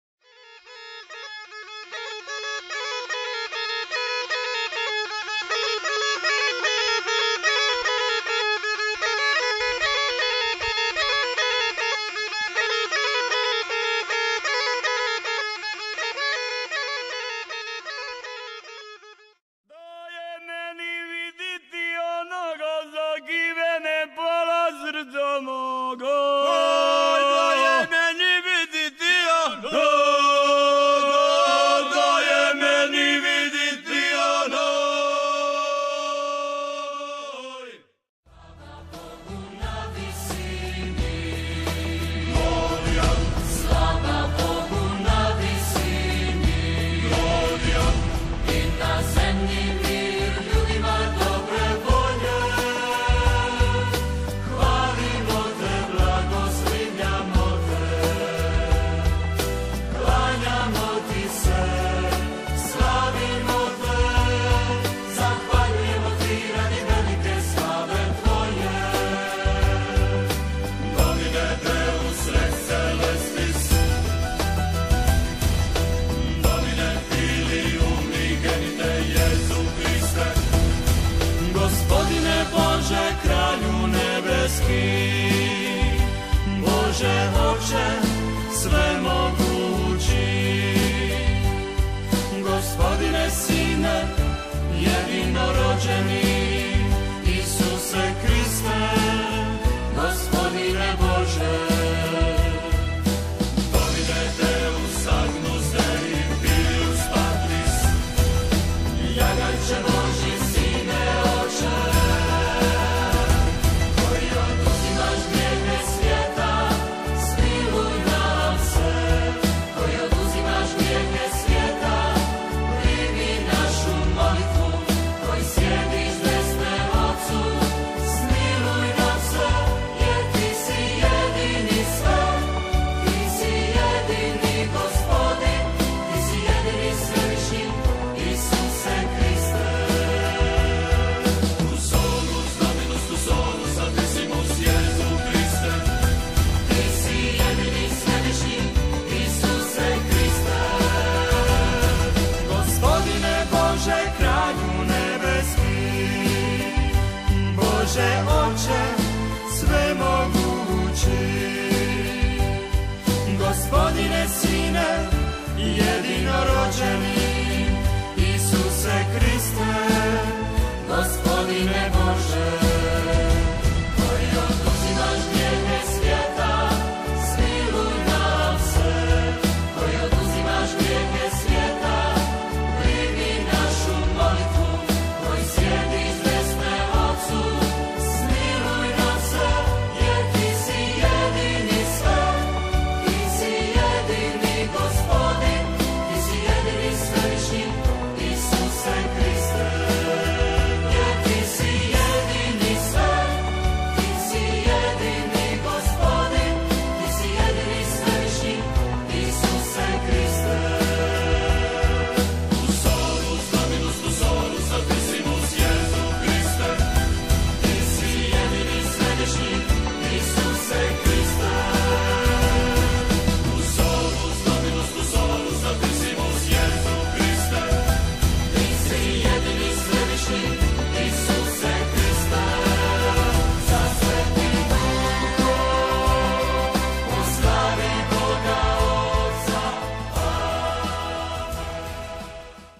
VISOVAC MONASTERY, N.PARK KRKA, CROATIA
Visovac je otočić na rijeci Krki, 10- ak km uzvodno od Skradina... na njemu se nalaze crkva i franjevački  samostan...  krasan krajolik i turistička atrakcija, jedno je od najljepših mjesta u RH